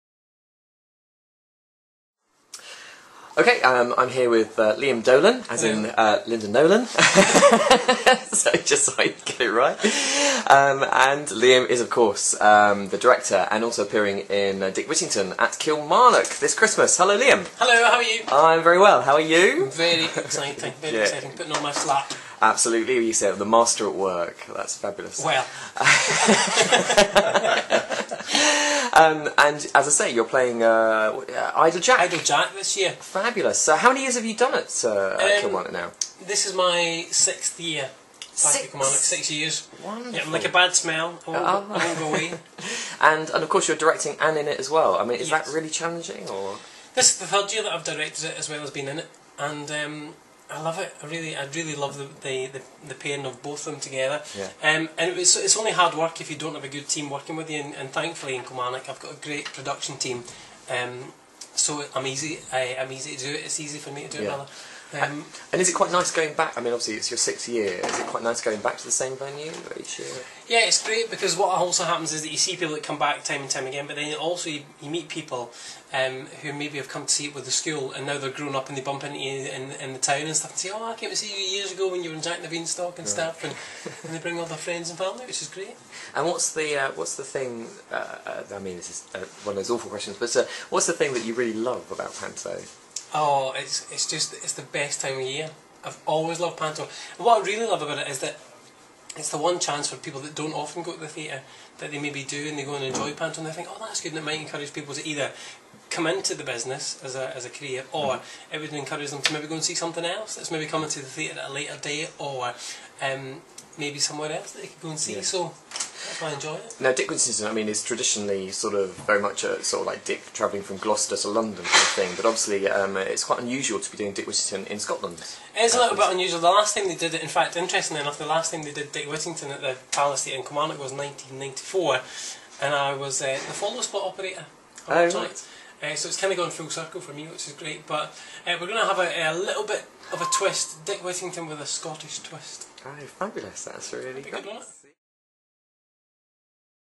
2010 Kilmarnock - Dick Whittington at the Palace Theatre - Meet Liam Dolan
September 2010 - Meet Liam Dolan, director and star of Dick Whittington at the Palace Theatre, Kilmarnock. The show is on from 25th November - 30th December 2010.